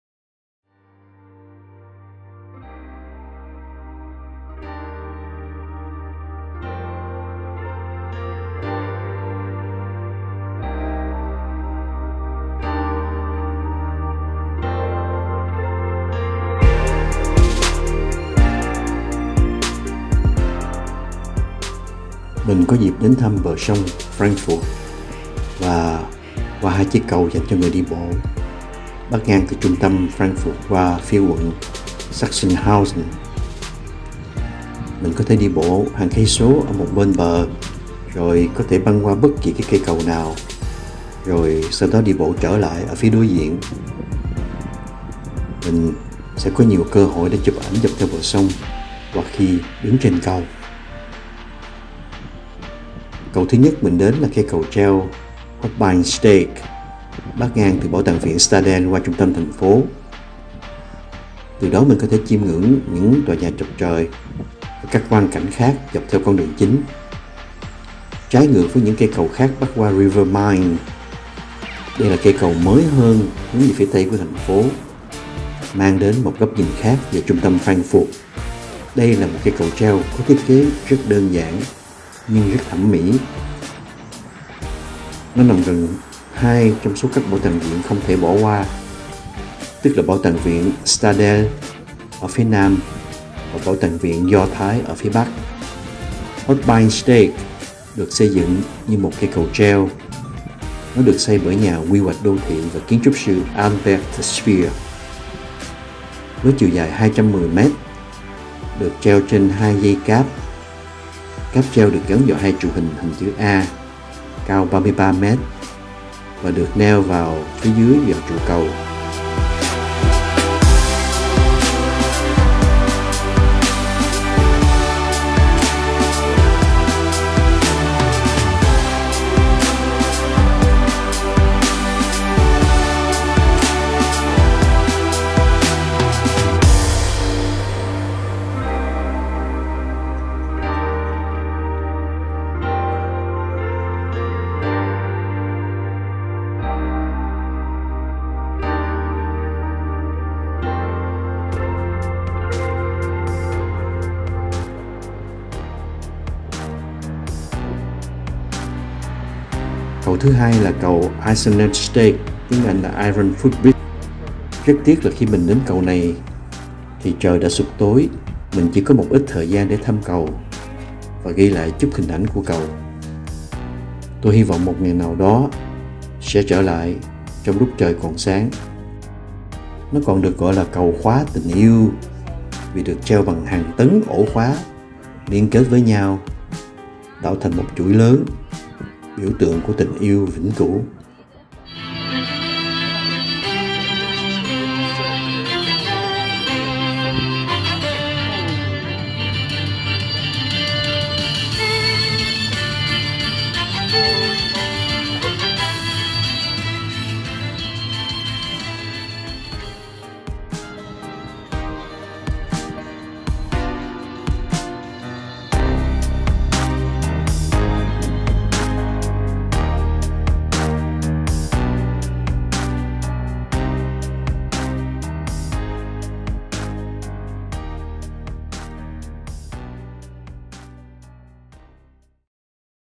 Cầu trên sông Main. Frankfurt.
I had the opportunity to visit the banks of the Frankfurt river and cross two pedestrian bridges spanning from the center of Frankfurt through the Sachsenhausen district. We can walk for kilometers on one side of the river, cross any bridge, and then walk back on the opposite side. I have many great photo opportunities along the river or standing on the bridge.

The first bridge I came to was the Holbeinsteg suspension bridge spanning from the Stadel museum through the city center. From there I can admire the skyscrapers and other views along the main road.
In contrast to the other bridges that span the River Main, this is a newer bridge facing the west of the city that offers a different perspective of central Frankfurt. This is a suspension bridge with a simple but aesthetic design that bends over the river. It is located near two of the must-see museums, the Staedel Museum to the South and the Jewish Museum to the North.

Holbeinsteg was built as a suspension bridge. It was built by urban planner and architect Albert Speer (Jr.) with a length of 210 meters, suspended by two cables. The cable car is attached to two A-shaped pylons, 33 meters high and anchored to the bridge piers by pairs of retaining cables.

The second bridge is the Eiserner Steg (English: Iron Footbridge). Unfortunately, when I reached this bridge, it was already dark. I only had a little time to visit the bridge and take some pictures of the bridge. I hope to return one day while it's still light.

One of the most popular photo spots with great views of Frankfurt's skyline. This famous iron and concrete bridge, crossed by more than 10,000 pedestrians every day, connects the city center and Römerberg with Sachsenhausen on the south bank of the River Main.

The neo-Gothic bridge was built in 1869 according to a plan by Peter Schmick, undergoing several alterations and modifications since then, the last taking place in 1993.

The Eiserner Steg (Iron Bridge) is a pedestrian bridge over the River Main connecting Frankfurt's Old Town with the Sachsenhausen district. It is also known as the "Love Lock Bridge" because it is hung with tons of locks linked together, forming a large chain, a symbol of "eternal love".

The first wrought iron bridge was built in 1868. It was replaced in 1911/1912 with a slightly larger cantilever bridge. The bridge is 170 m long and consists of riveted steel trusses with two piers. The bridge was blown up by the Wehrmacht (It was the unified armed forces of Nazi Germany) during the last days of World War II, but it was rebuilt shortly thereafter in 1946. It was renovated. Completely built in 1993, two lifts were later added to facilitate wheelchair accessibility.